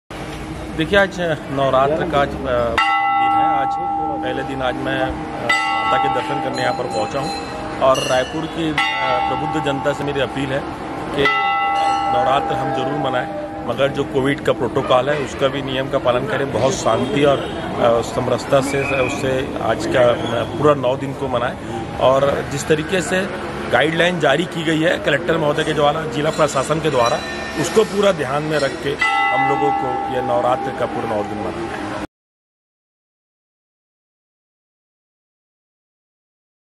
महापौर एजाज ढेबर की राजधानी वासियों से अपील शांति और सद्भावना से मनाएं नवरात्र पर्व |raipur CG news
date : 07/10/21
location : mahamaya mandir, raipur (CG)